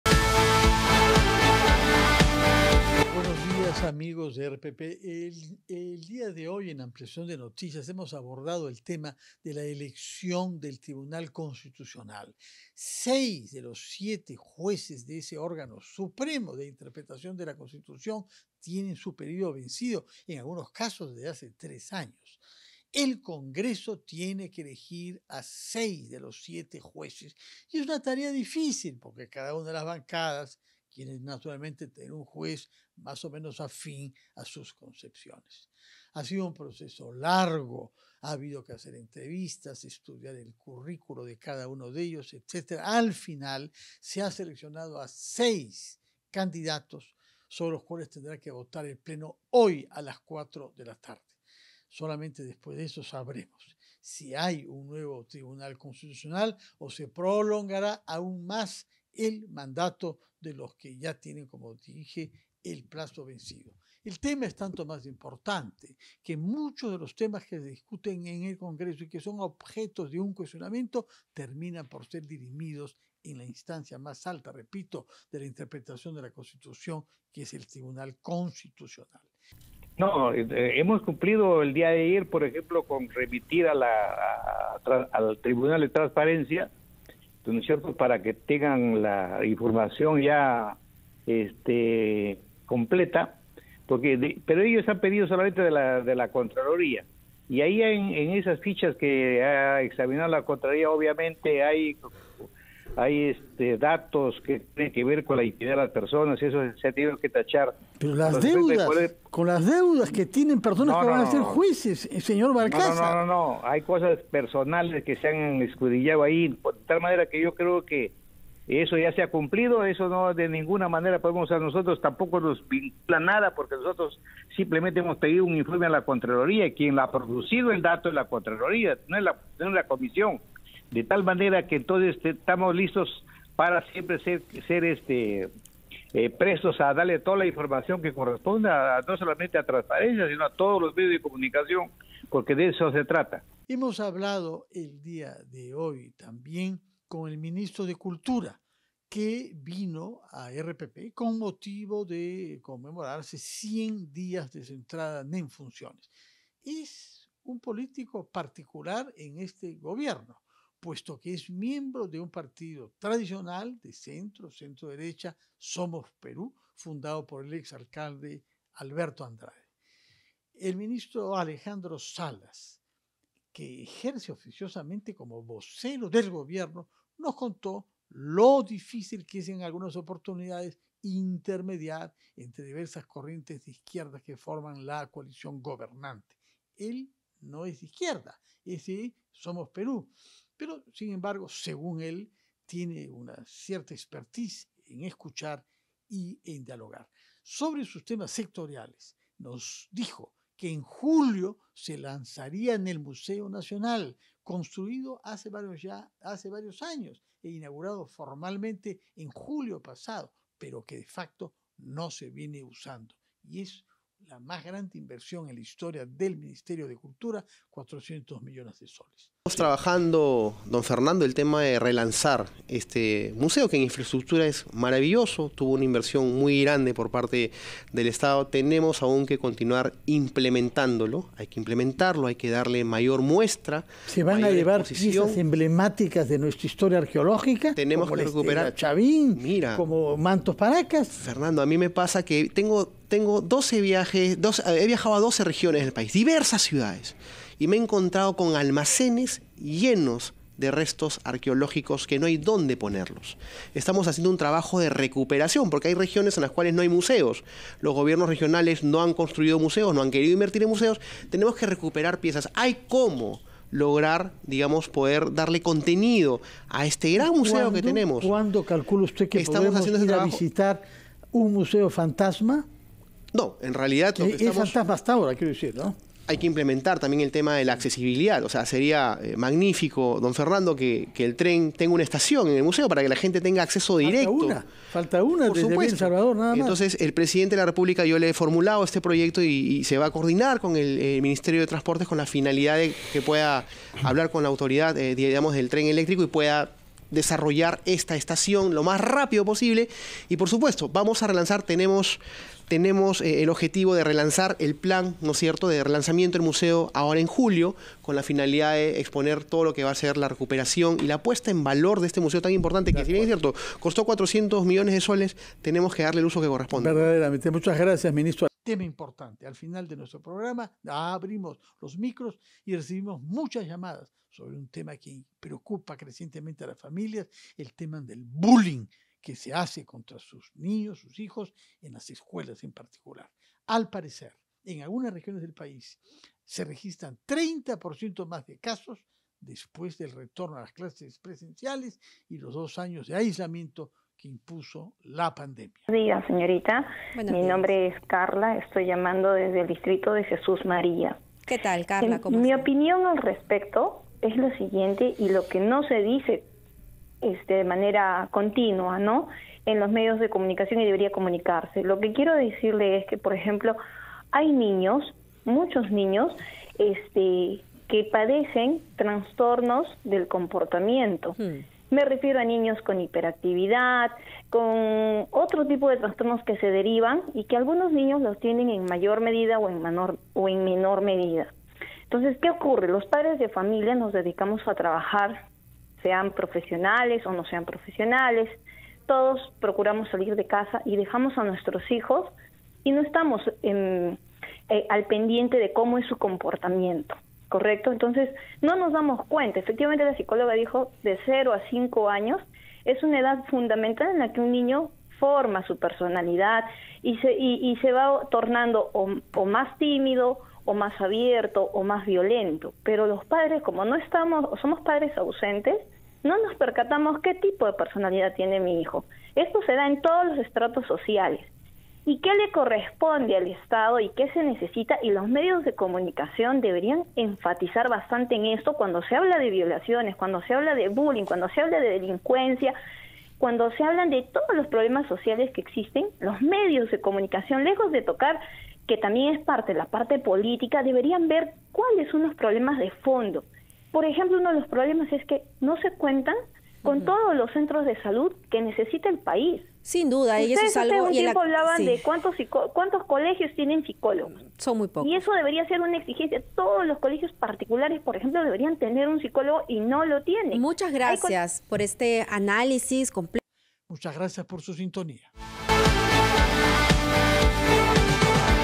🔴 Bullying en Perú: "No nos percatamos qué tipo de personalidad tienen nuestros hijos"
En las últimas semanas, se han reportado varios casos de bullying en el país y los padres están preocupados por esto. RPP Noticias abrió los micrófonos para que estos pudieran expresar sus opiniones.